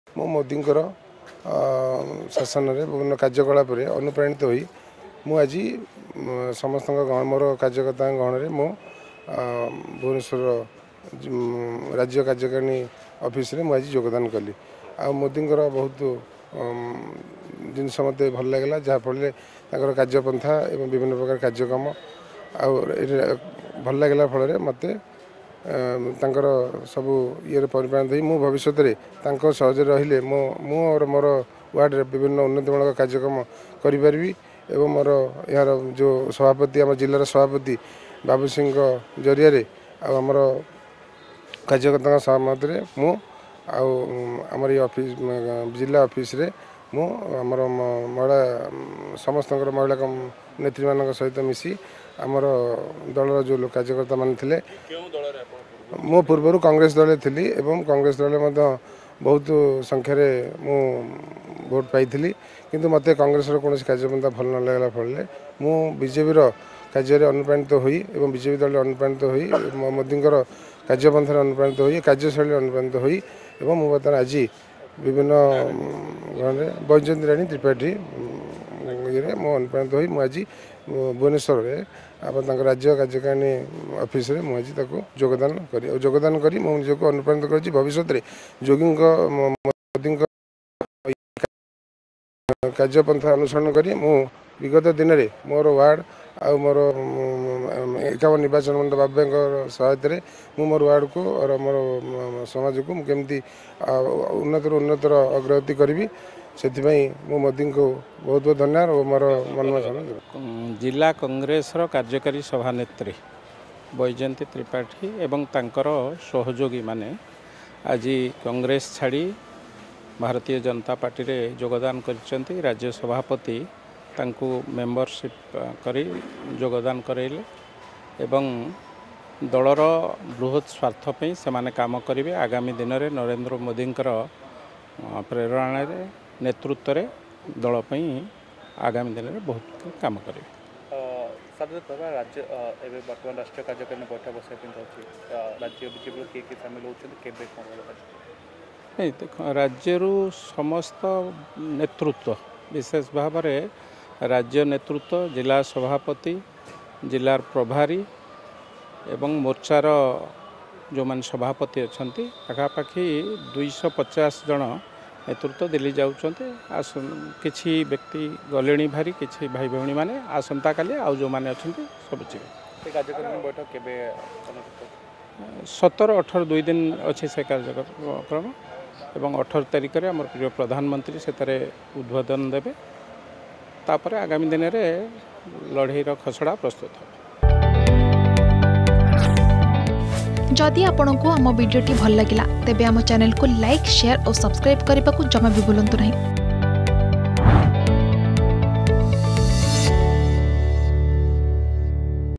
ବିଜେପିରେ ମିଶିଲେ କଂଗ୍ରେସ କର୍ମୀ || BJP || Mishrana Parba || Bhubaneswar
ବିଜେପିରେ ମିଶିଲେ ଭୁବନେଶ୍ୱର ଜିଲ୍ଲା କଂଗ୍ରେସର କାର୍ଯ୍ୟକାରୀ ସଭାନେତ୍ରୀ ବୈଜୟନ୍ତି ତ୍ରିପାଠୀ ଏବଂ ତାଙ୍କ ସମର୍ଥକ ।